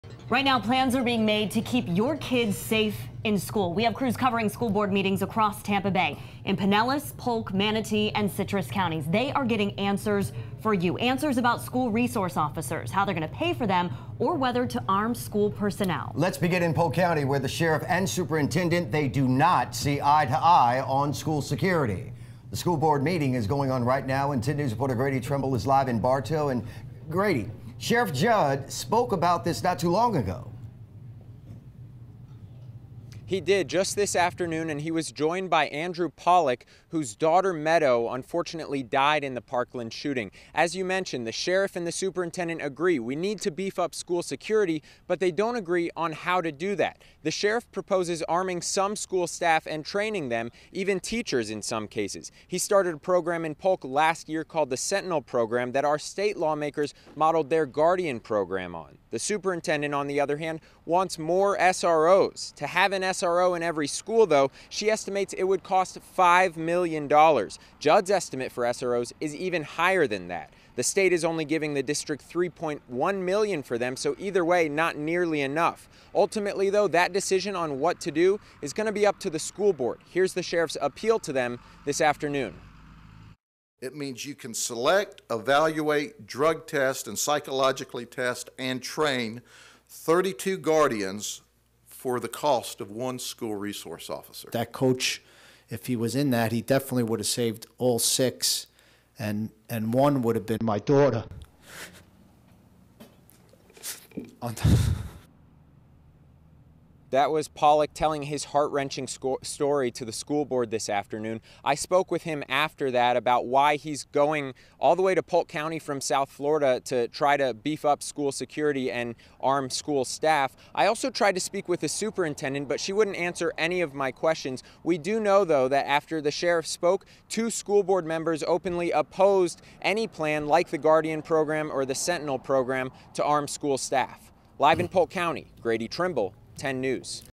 Sheriff Judd explains Sentinel program to Polk school board
Polk County Sheriff Grady Judd explains the ins and outs of the Sentinel program to Polk school board members.